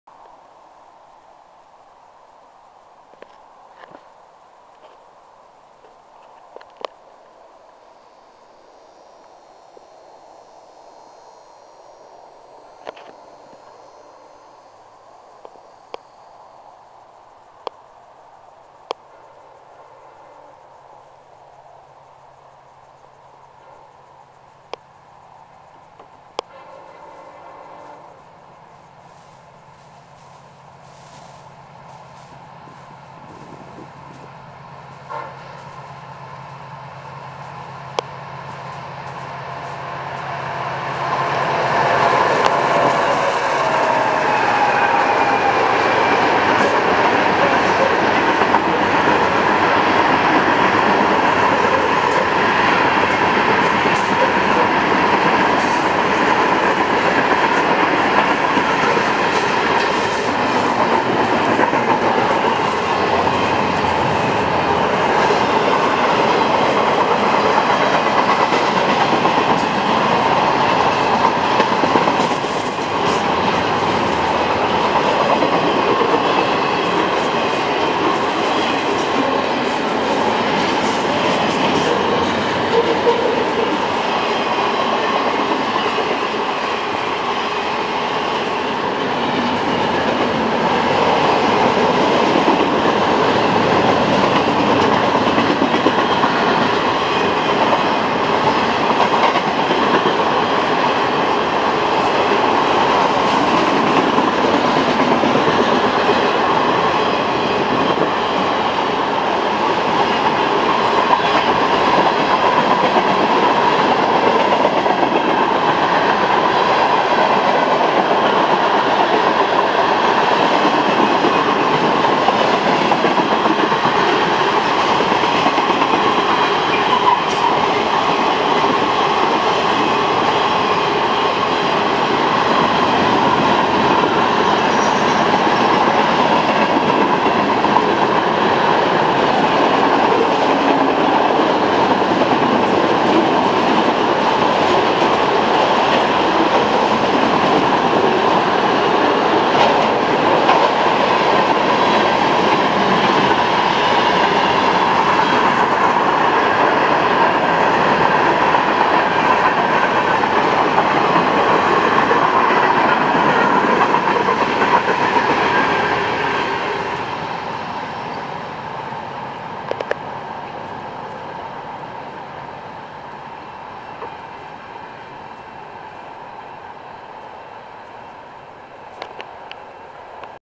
FEC202 Sept 2, 2011
RedPearlBlue 107 leading this train at Limited Speed toward the meet in Micco. Love the blue doublestacks, lot of work cars in this train and the taggers have added color to some of them. A coal car in there, too, EoT past Caboose Junction at 5:58PM.